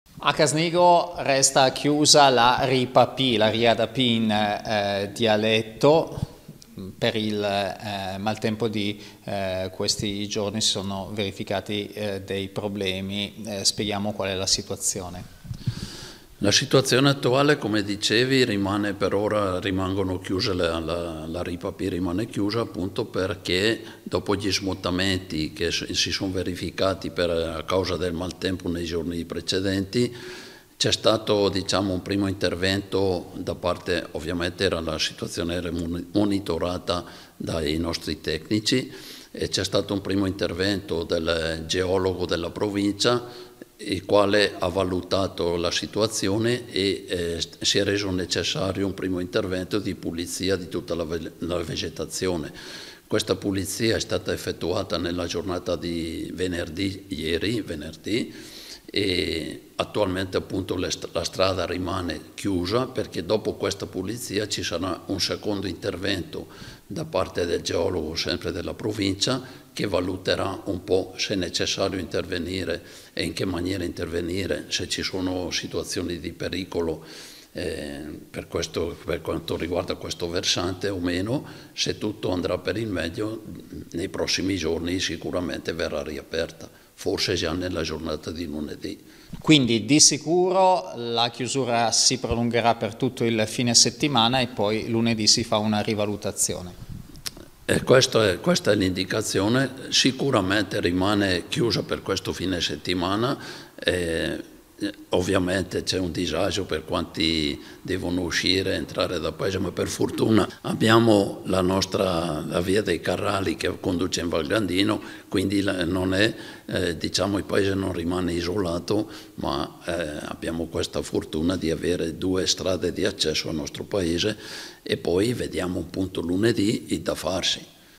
Casnigo, resta chiusa la Ria da Pì
Un fine settimana senza la Ria da Pì, a Casnigo. Il primo tratto della provinciale 45, che permette di accedere al paese dalla rotatoria lungo la Statale 671, resta infatti chiuso al traffico. Un altro effetto del maltempo che ha colpito la Val Seriana nei giorni scorsi.